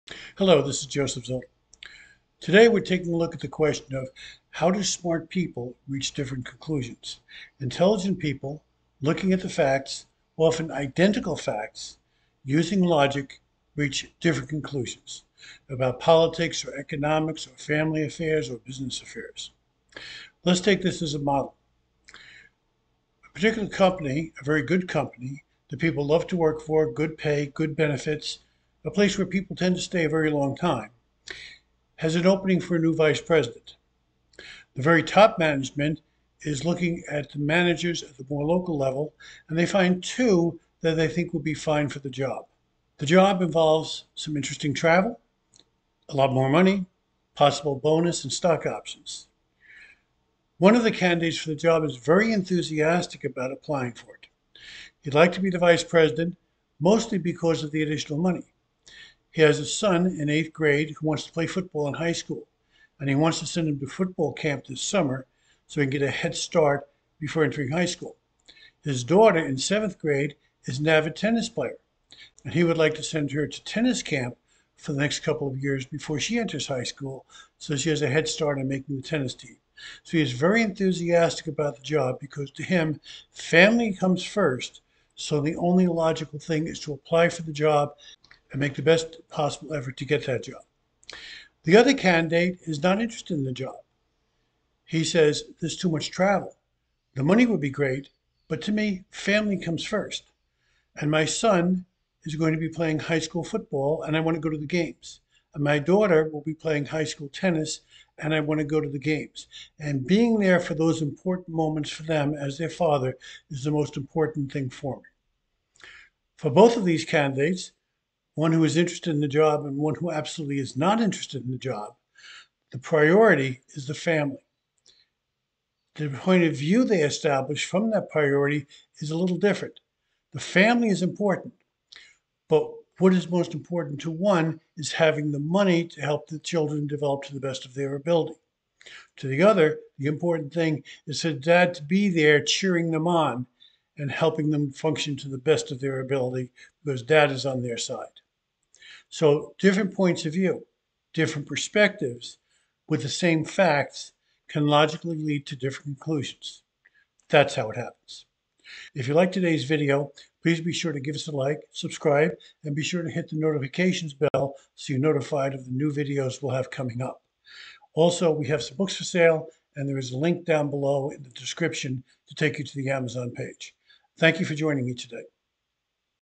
How do smart people reach different conclusions? Important information!
How do smart, intelligent people reach different conclusions, often with the same facts and thinking logically? This can involve decisions on politics, social issues, family matters, business matters, and more. We'll explore that question today,

Links:
FREE access to Joseph Zodl's Favorite Essays, Poems, and Stories from various authors